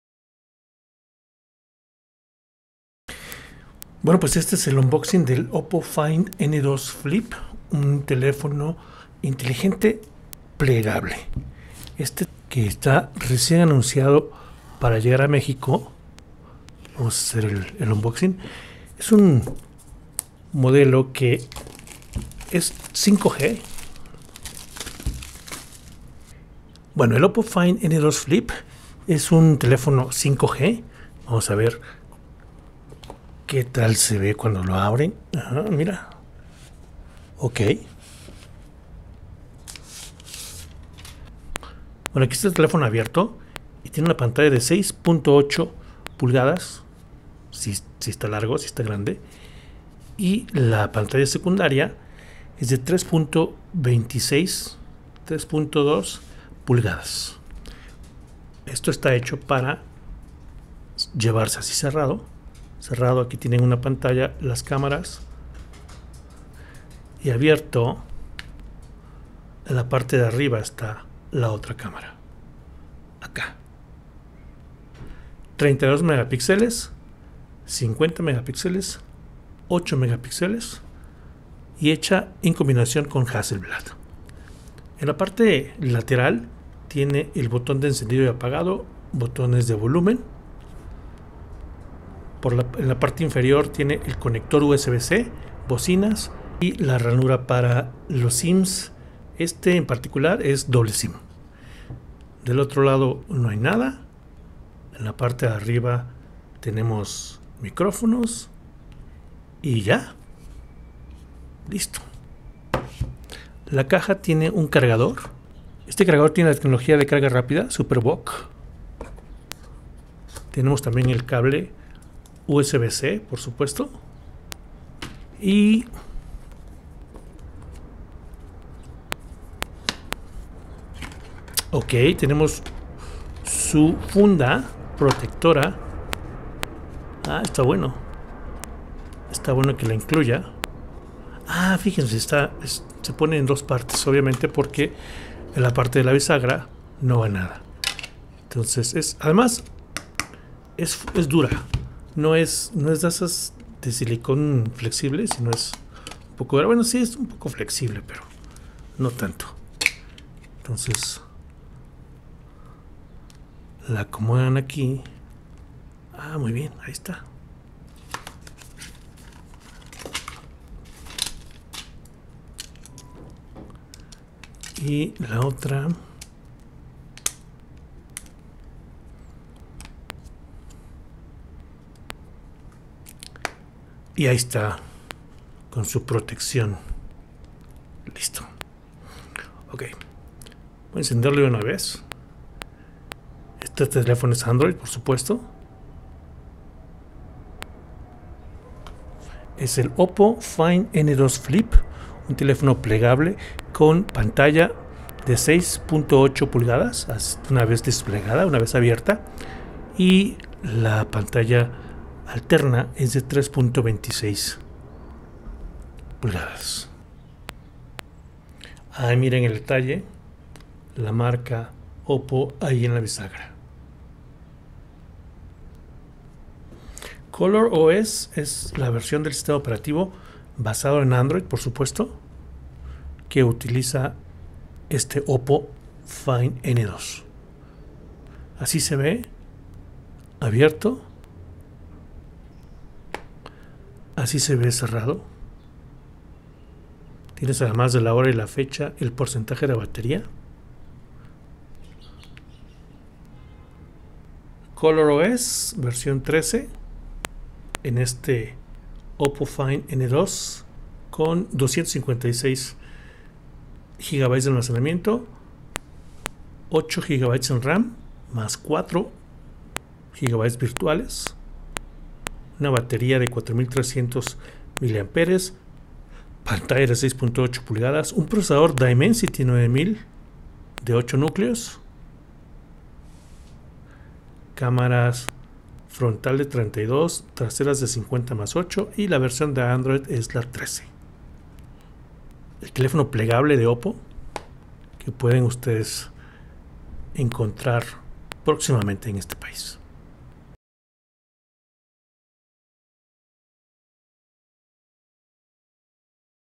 Unboxing del Oppo Find N2 Flip
Recién presentado en el MWC de Barcelona, el Oppo Find N2 Flip es un smartphone plegable que llegará este año a México. Aquí les hago el unboxing para que lo puedan conocer a detalle.